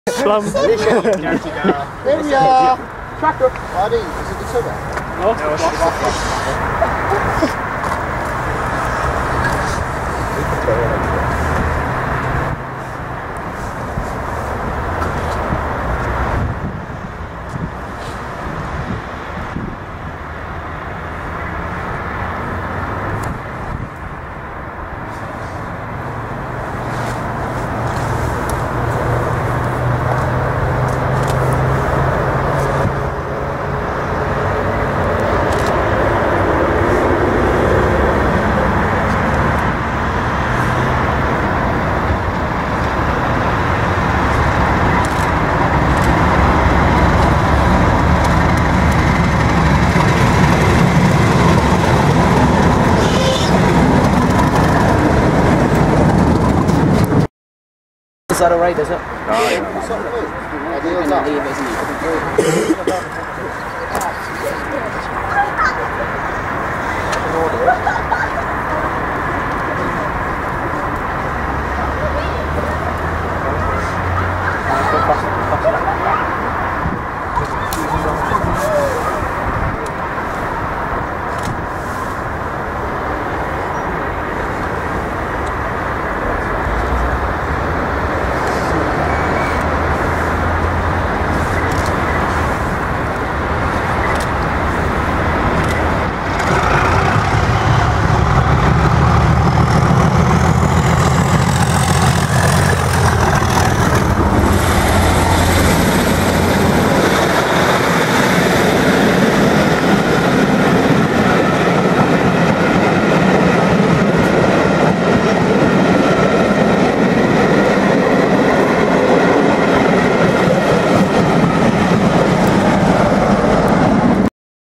DRS Class 37 and 47 locos at Metrocentre 37608 and 37402 then 37607 and 37409 - 29th March 2013
In a short space of time, just 5 minutes, four Direct Rail Services (DRS) Class 37s and one DRS Class 47 passed Metrocentre, Gateshead on the 29th March 2013.
The first video we see is a fairly regular service that runs between Seaton-on-Tees to Sellafield with Nuclear flasks working 6M60 with DRS Class 37s No. 37606 and 37402
We then see 'The Easter Highlander' Railtour with 1Z31 from Exeter heading Paisley with DRS locos No. 37607 and 37409 in charge with 47853 on the rear.